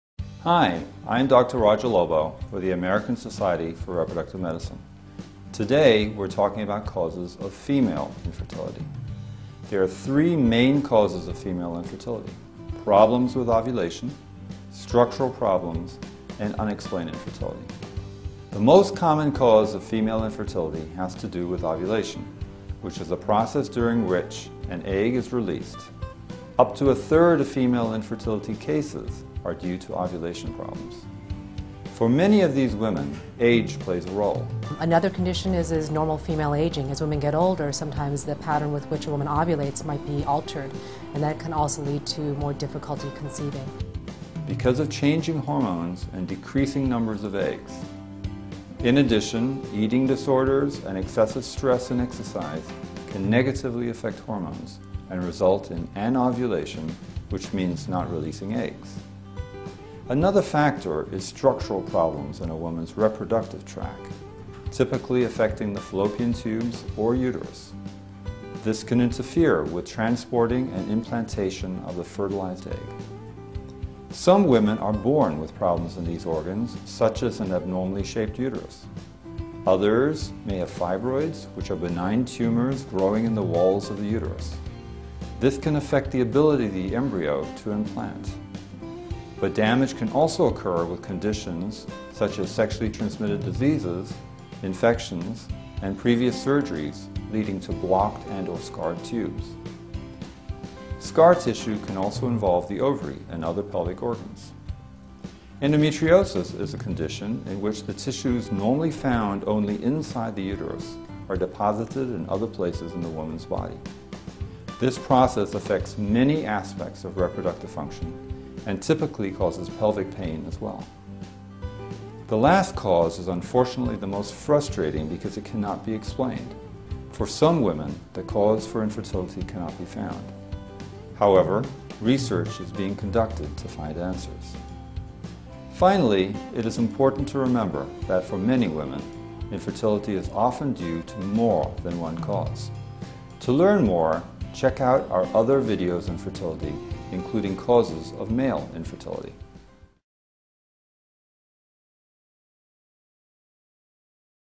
Causes of Female Infertility
Dr. Roger Lobo, of the American Society for Reproductive Medicine explains the causes of female infertility.